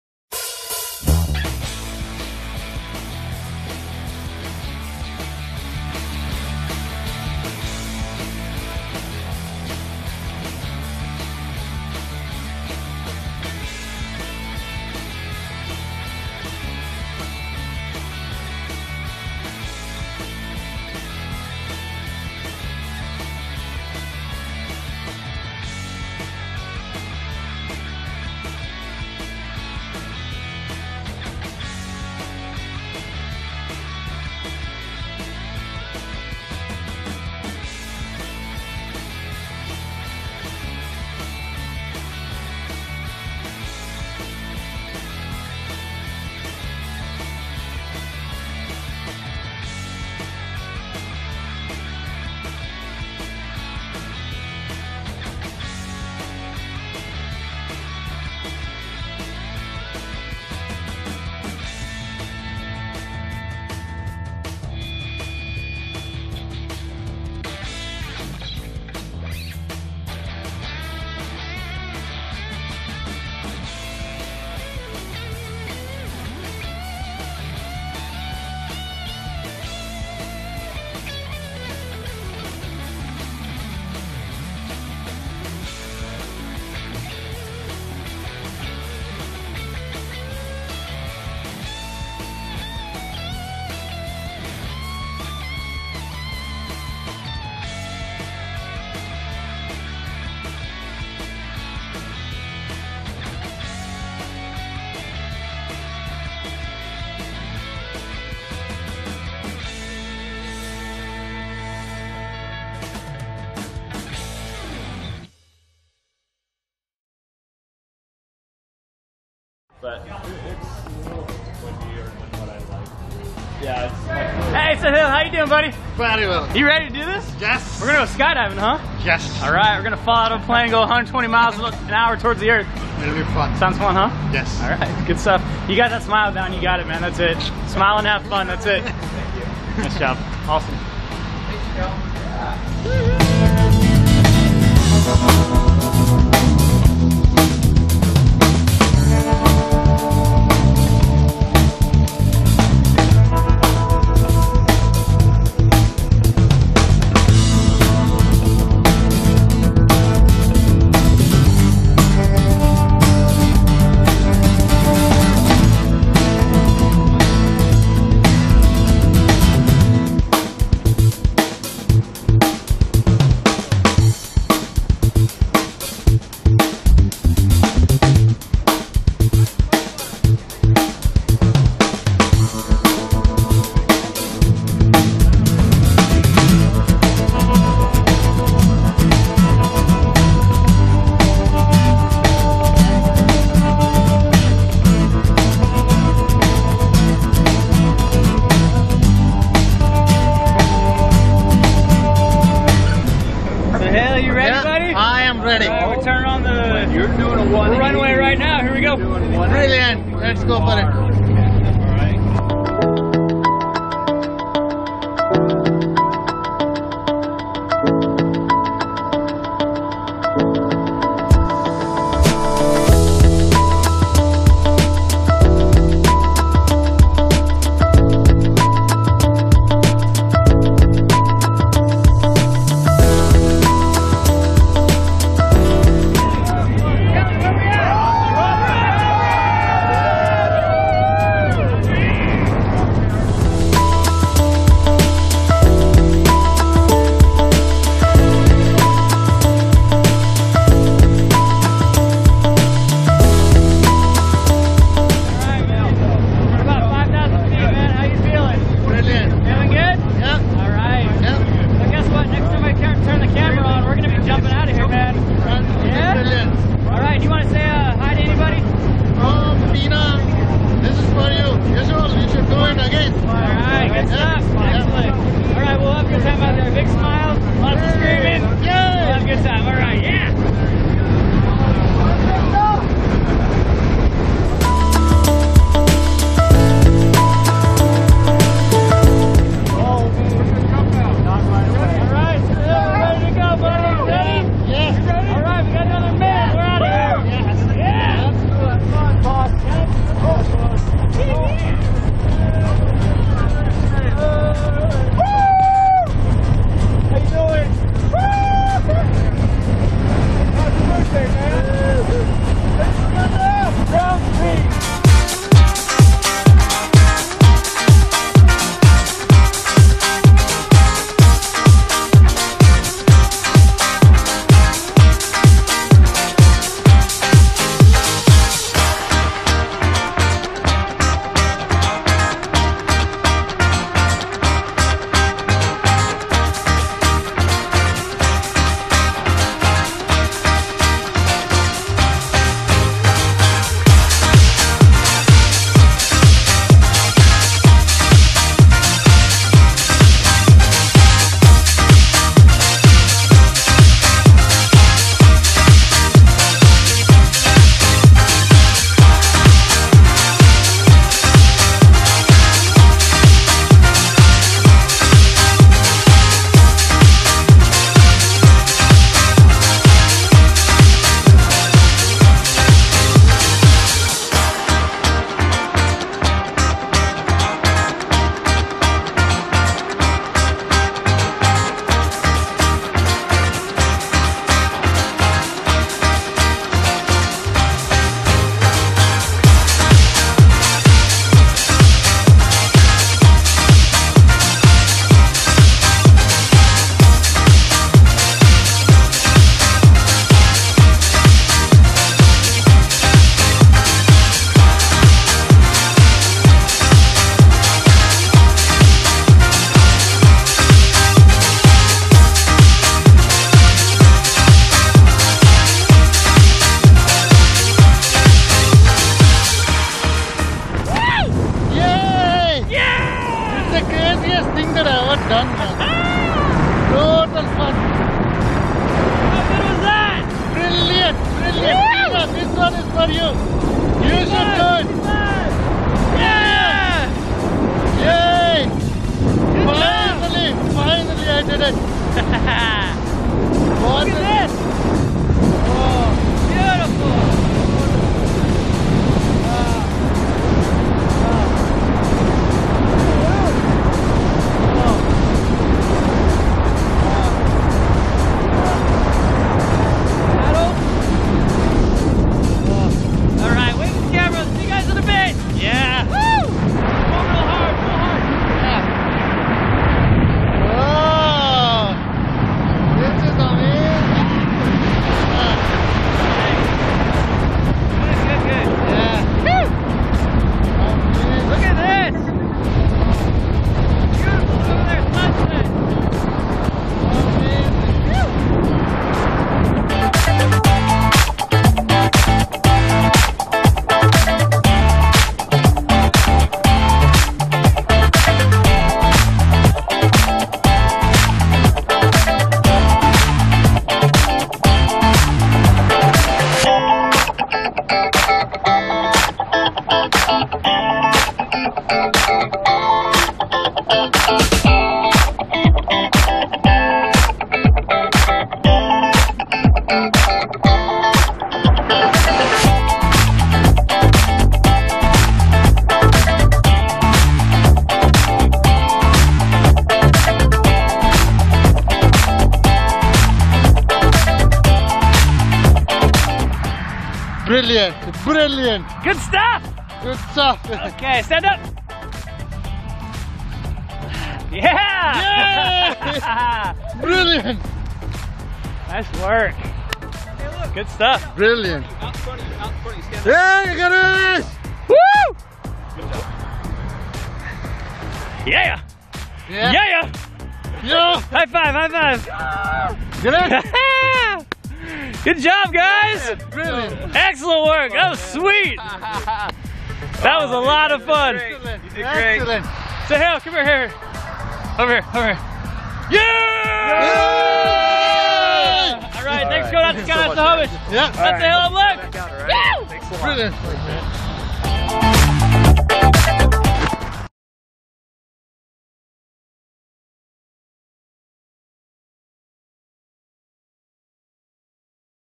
Sahil Sagar's Tandem skydive!
Sahil Sagar's tandem skydive on April 12, 2014 with Luke "Skywalker" Nevin.